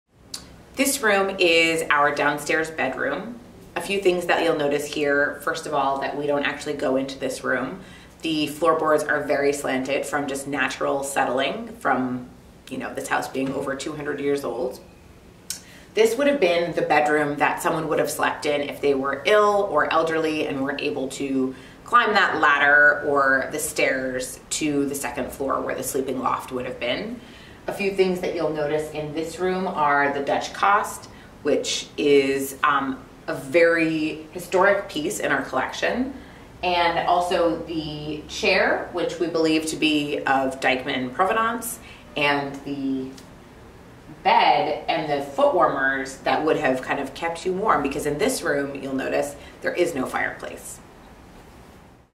Colonial Bedroom Tour at the Dyckman Farmhouse Museum
The Dyckman Farmhouse Museum only has one bedroom on the first floor of the house. Meredith Horsford, Executive Director of the Historic House Trust of NYC and former Director of the Dyckman Farmhouse Museum, shares a little bit about this space.

The upstairs of the Dyckman Farmhouse would have been one big sleeping loft accessible by only a ladder during the time the Dyckman's lived in the home. The downstairs bedroom would have been used for those who were sick, elderly, pregnant, or could not climb the ladder for whatever reason. Some notable objects in this room include a Dutch Kast, a Dyckman armchair, and a food warmer.